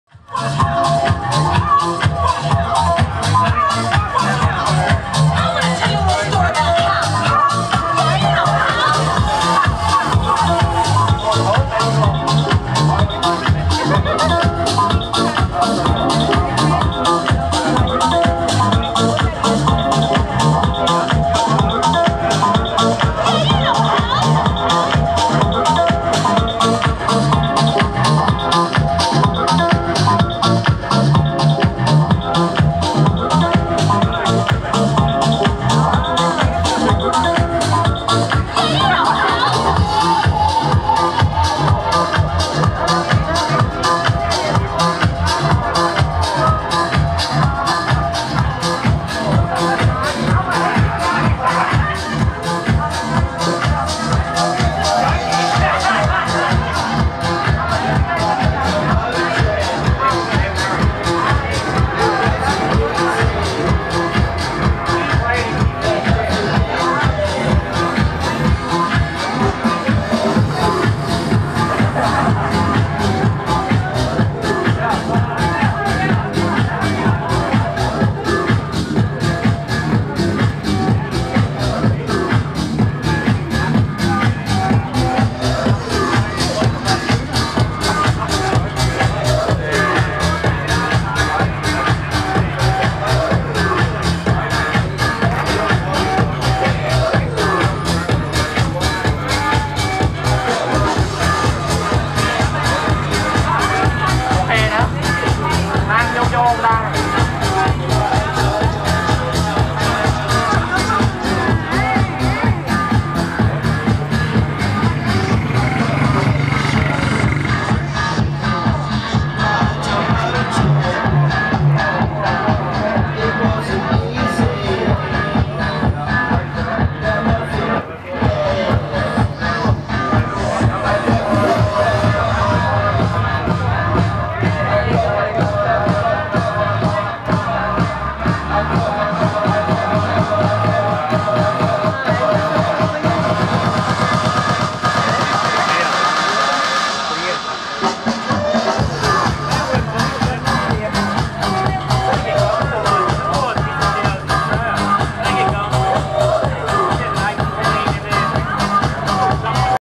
dj.ToBi at on street bar.
On street bar Inspirational street bar with daily changing art and decoration. Perfect to meet people, have a drink and chill out after a long lazy day at the beach or to start your night out .6:pm-12:00pm